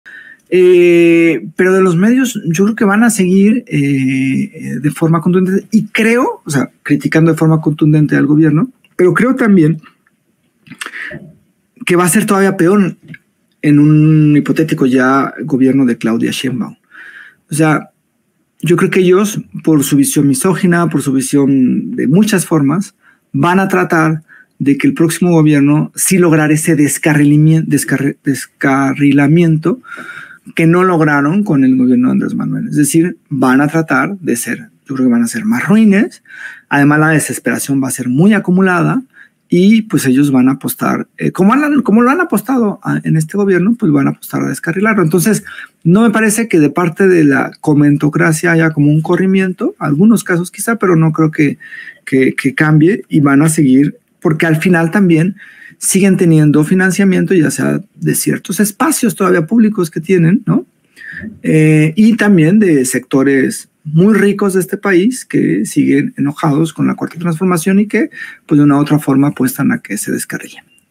CON SHEINBAUM LOS MEDIOS TRATARÁN DE LOGRAR EL DESCARRILAMIENTO QUE NO PUDIERON CON AMLO
Los medios por su visión misógina van a tratar de descarrilar el gobierno de Sheinbaum, lo que no lograron con López Obrador. Serán más ruines y la desesperación estará más acumulada.
JORGE GÓMEZ NAREDO: Analista Político